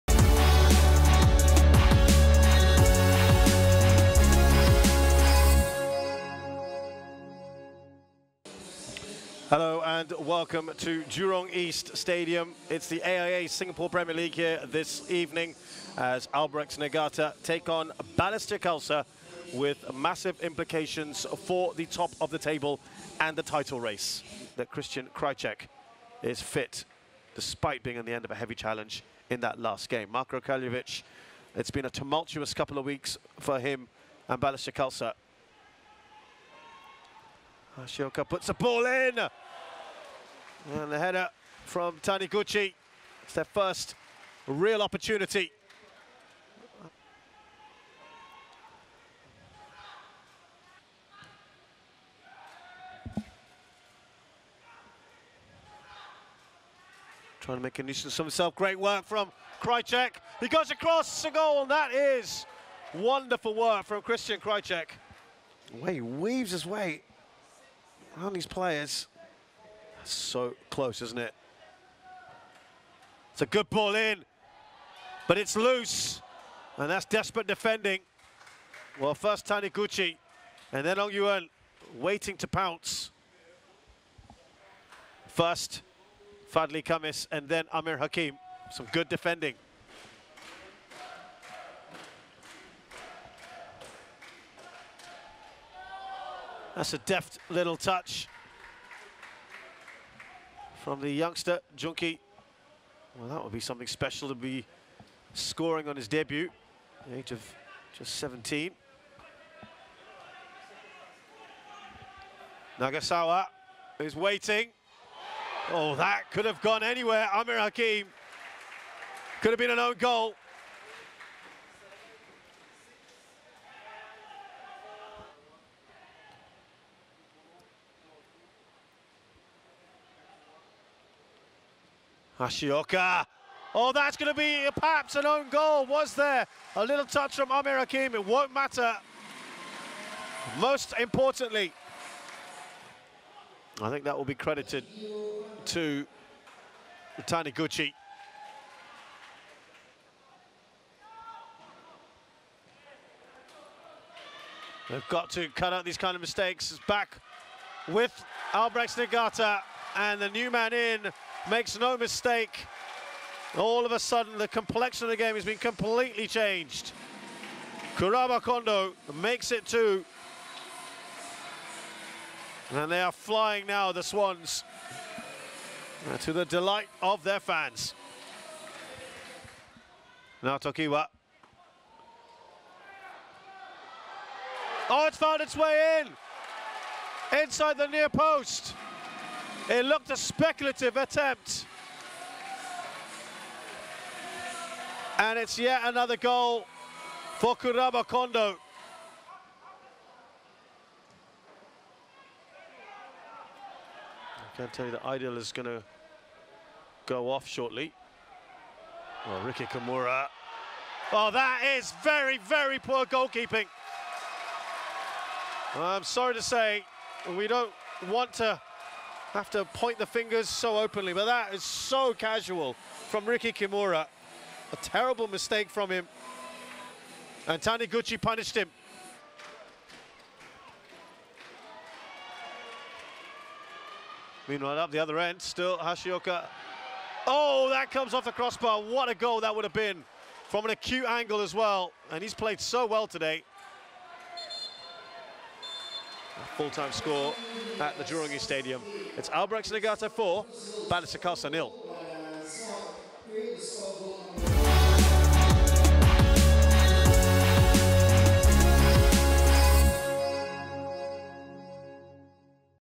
2021 AIA Singapore Premier League: Albirex Niigata (S) vs Balestier Khalsa
Date: 28 August 2021
Venue: Jurong East Stadium
Score: 4-0 (Ryoya Taniguchi 47', 77', Kuraba Kondo 50', 64')